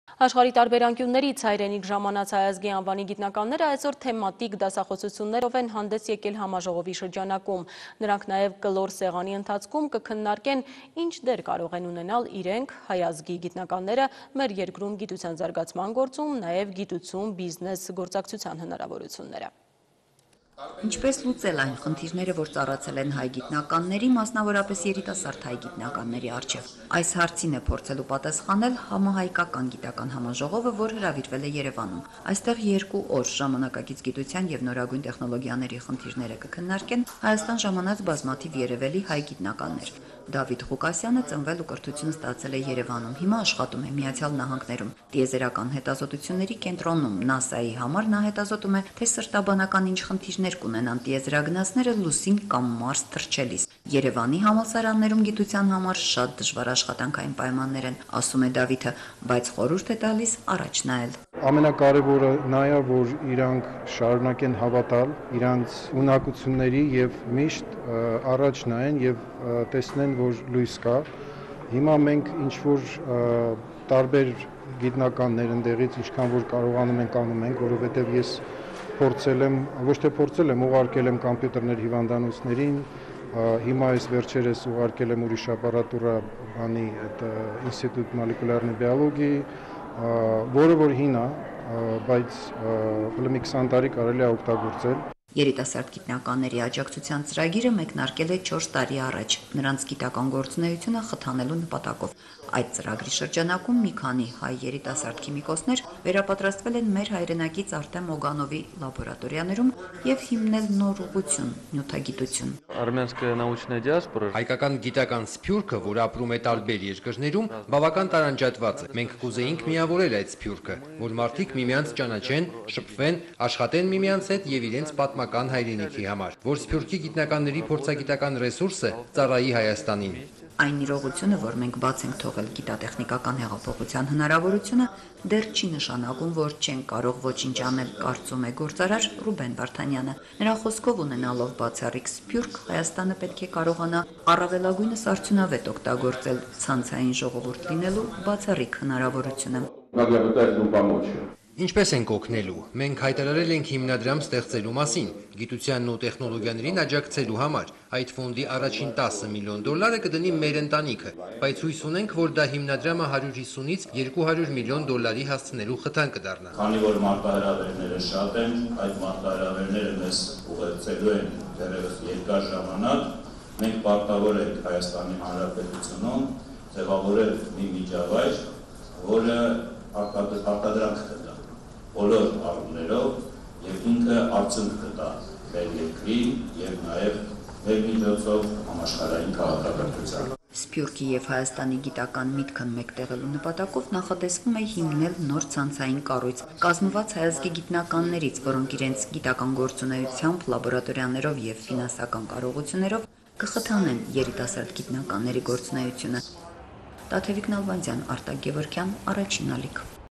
Ruben Vardanyan addressed the science conference in Yerevan
IDeA co-founder Ruben Vardanyan addressed the science conference in Yerevan on September 22, 2016 and talked about FAST Foundation. Coverage: H1